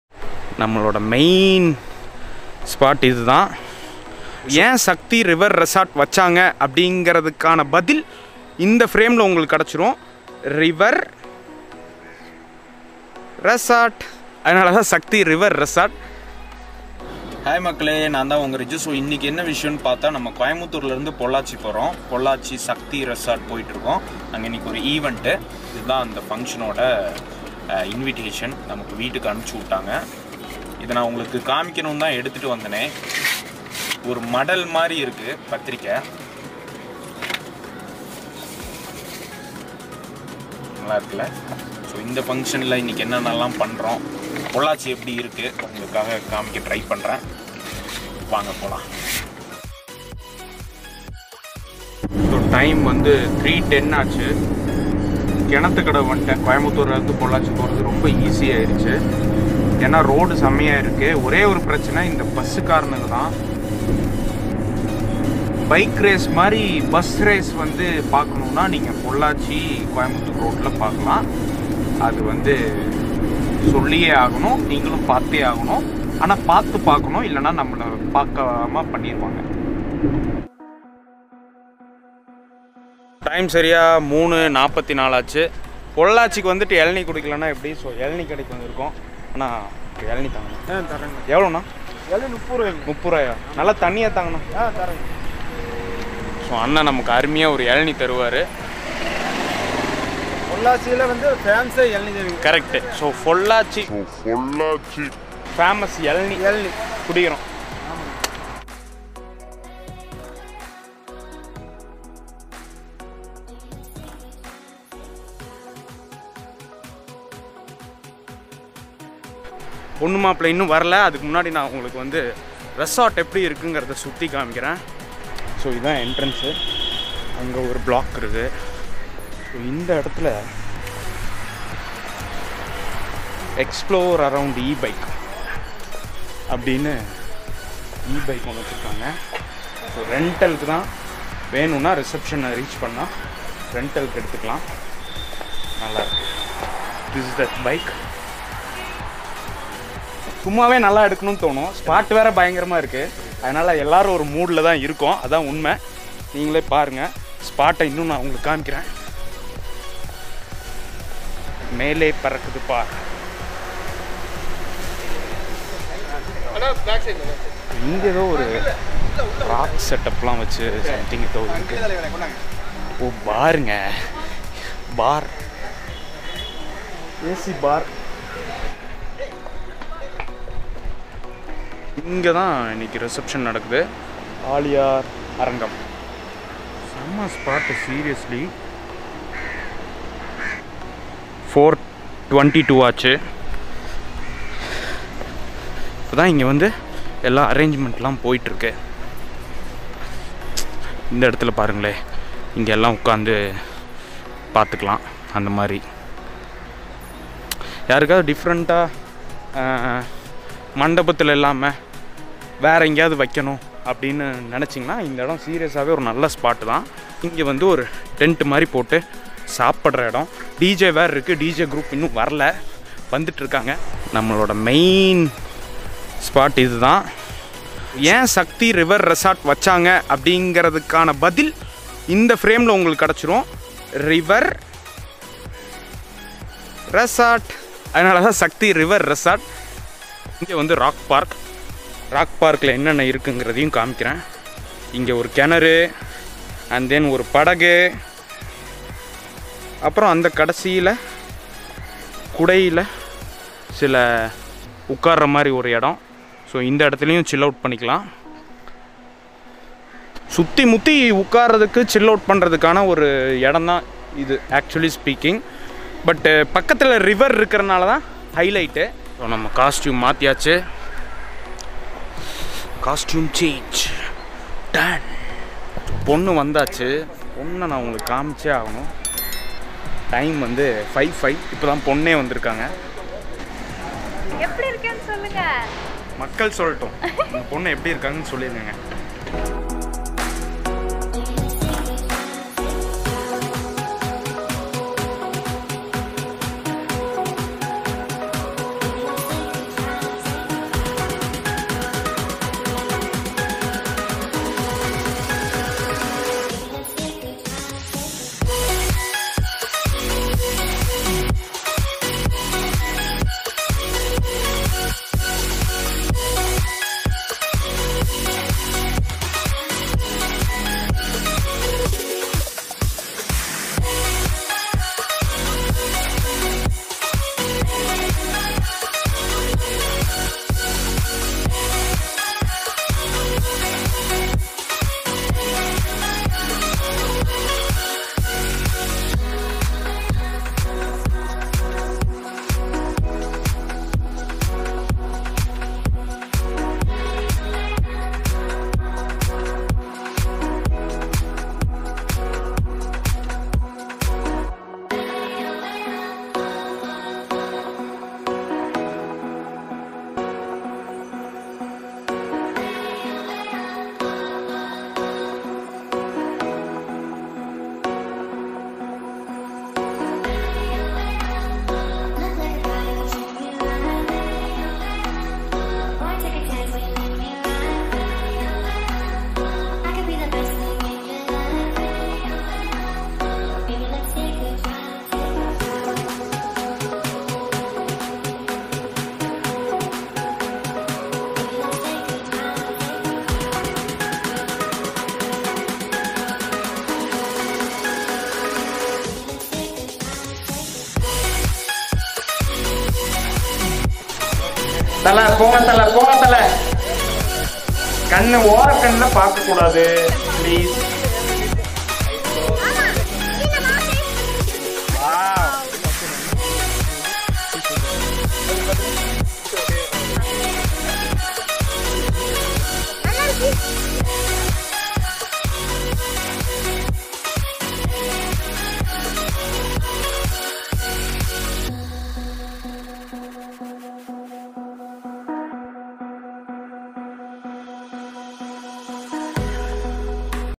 Resort for wedding | Sakthi River Resort | Weekend spot | Coimbatore to Pollachi | Settufittu | Riju
Hi Makkale,

In This video we will explore the Sakthi river resort, Pollachi. I travelled from Coimbatore to Pollachi for an event @ Sakthi river resort & I felt it’s a perfect river resort for wedding ceremony. Just 1 hour travel from Coimbatore to the Sakthi river resort Pollachi, & it’s a perfect Weekend spot for chill out around Coimbatore.
Congrats Shiram ❤️ Suganya for their new Beginning!!

Peace..!!

Life la yellame Settu Fittu

Thanks for the Music by Amidst